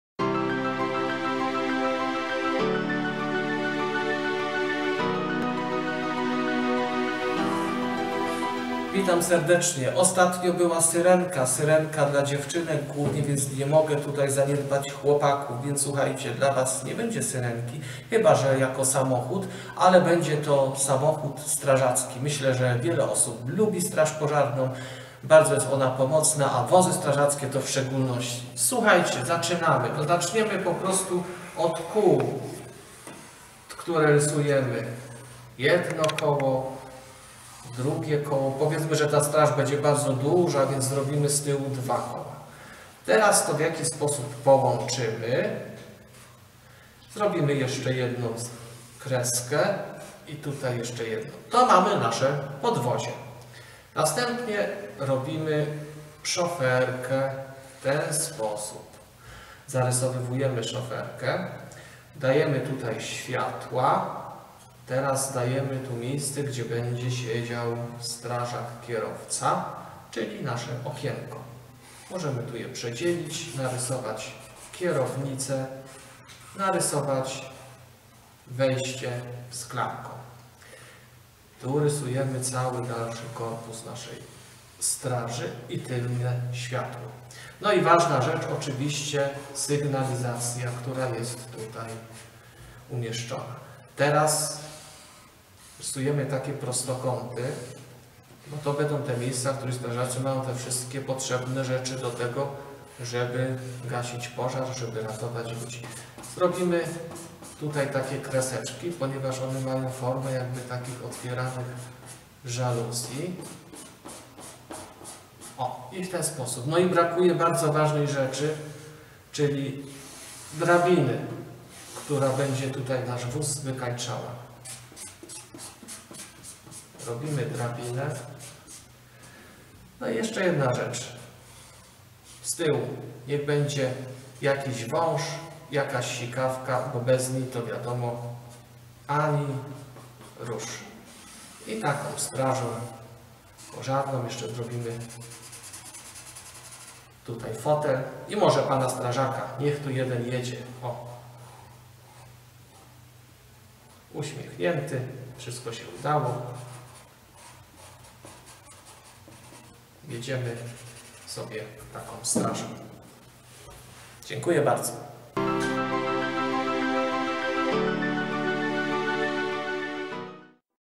Jak narysować wóz strażacki?
Krok po kroku, prosto i szybko!
Rysuje Maciej Paździor, instruktor zajęć plastycznych.
Cykl powstał we współpracy z Miejskim Domem Kultury w Mikołowie.